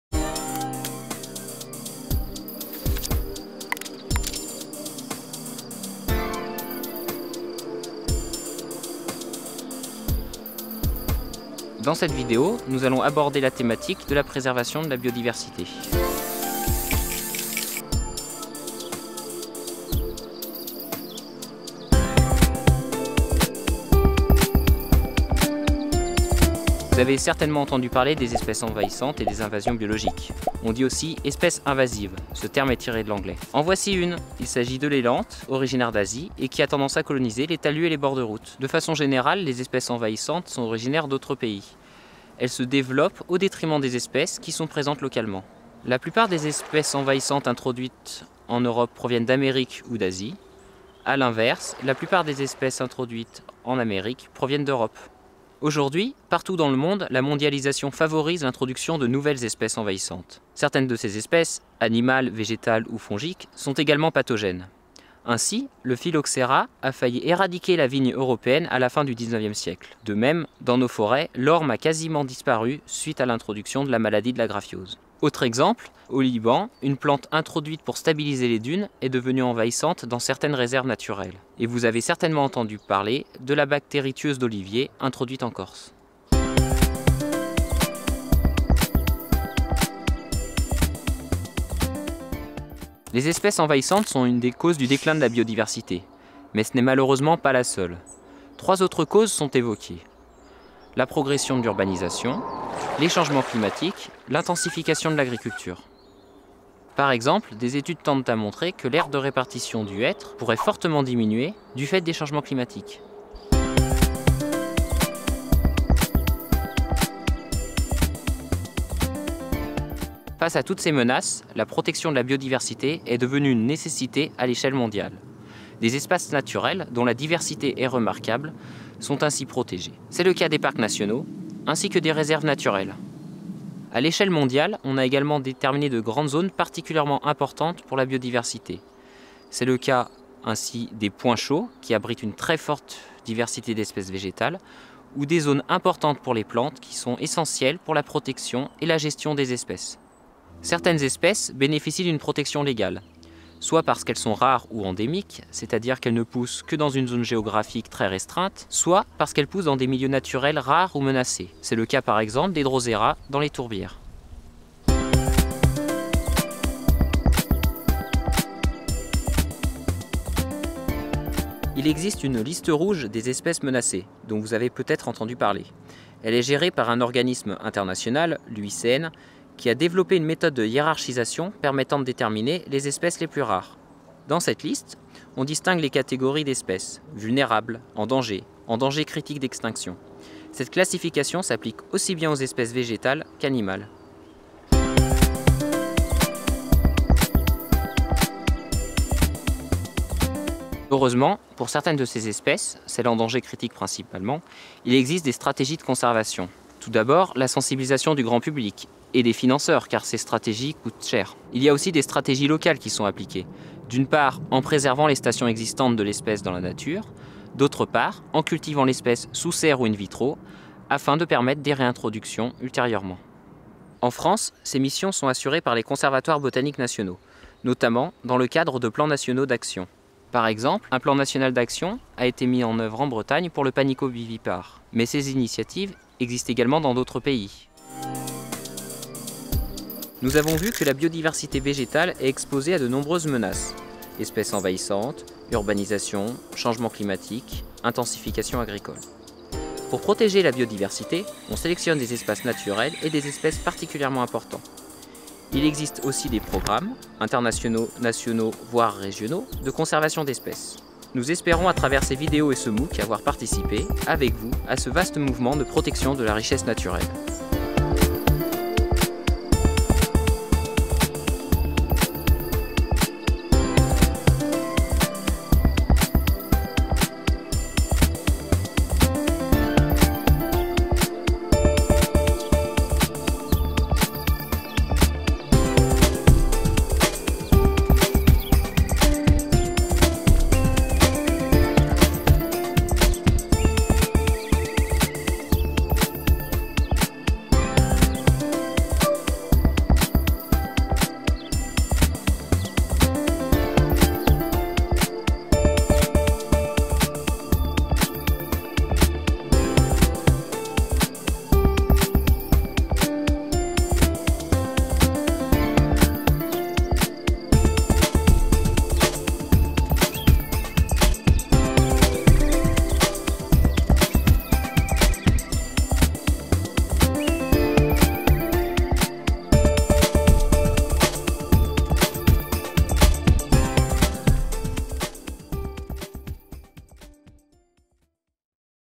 Séquence 6 sujet 3 : Protéger la biodiversité
Après l'archivage et l'éthique, cette vidéo du MOOC Botanique est consacrée à la protection de la biodiversité.